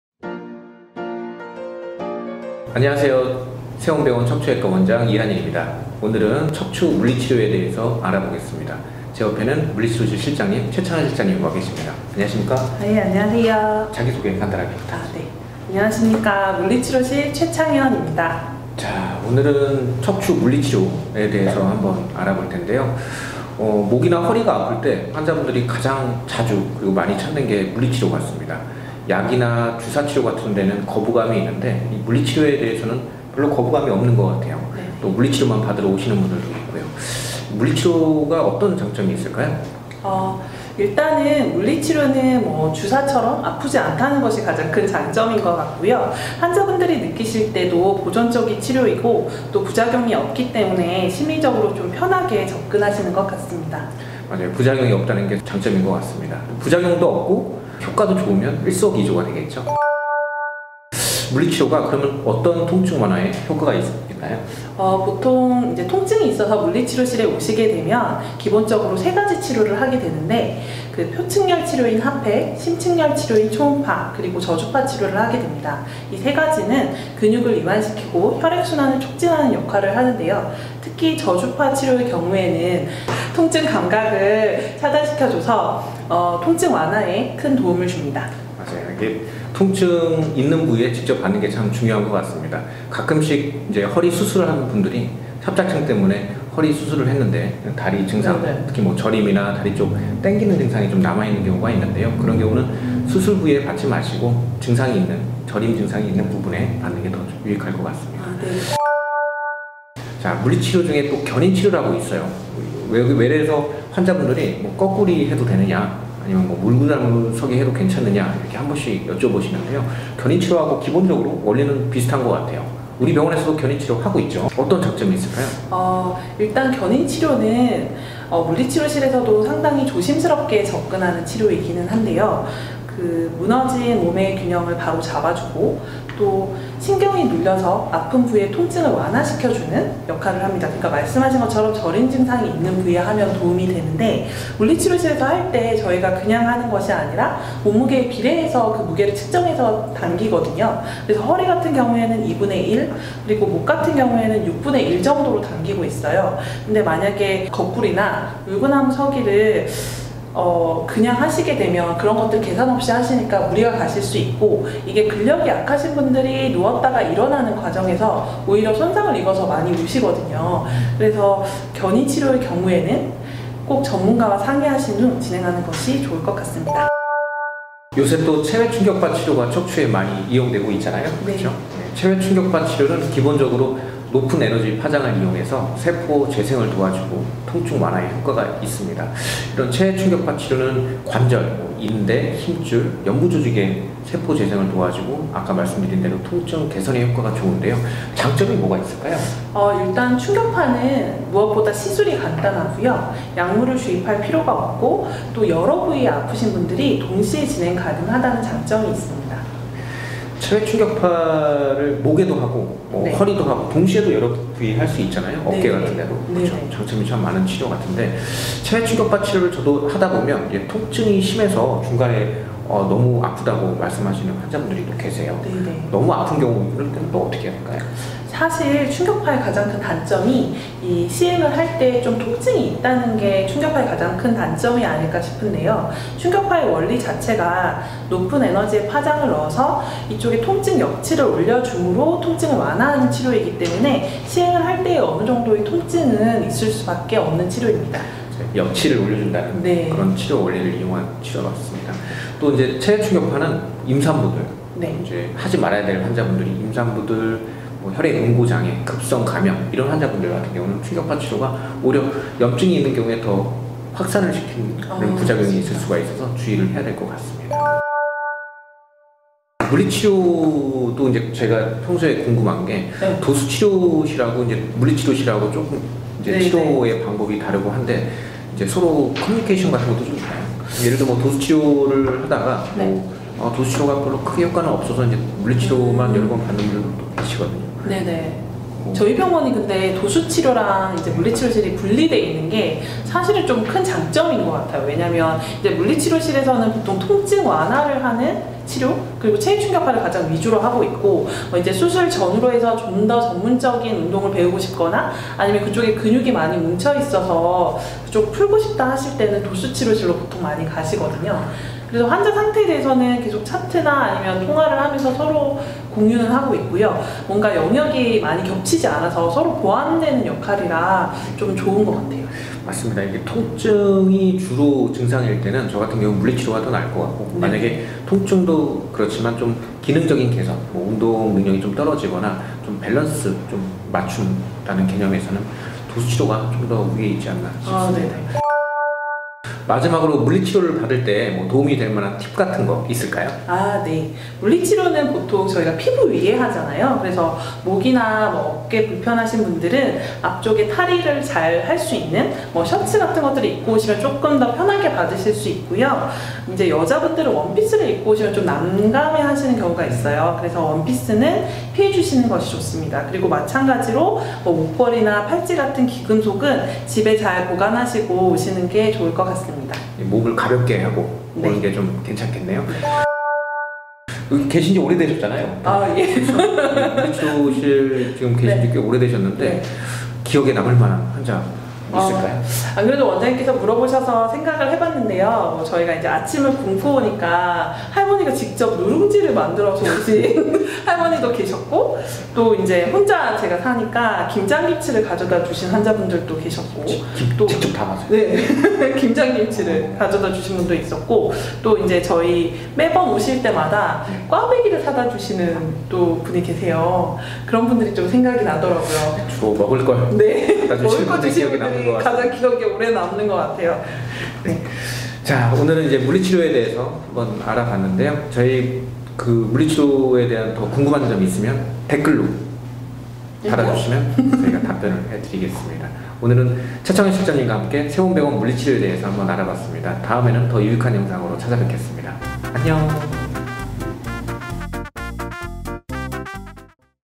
물리치료 궁금증 모아봤습니다! (기본 3가지 치료, 견인치료, 체외충격파)
궁금했던 물리치료 궁금증을 풀어봅시다!
새움병원 척추클리닉 이한일 원장 & 물리치료실 최창연실장

00:39 물리치료의 장점
01:00 물리치료가 어떤 통증완화에 효과가 있나요?
01:49 견인 치료는 어떤 치료? 거꾸리 해도 되나요?
02:57 체외충격파는 어떤 치료인가요?
04:28 새움병원 물리치료와 도수치료!
05:38 물리치료를 잘 받기 위한 조언!
06:13 기억에 남는 환자가 있다면?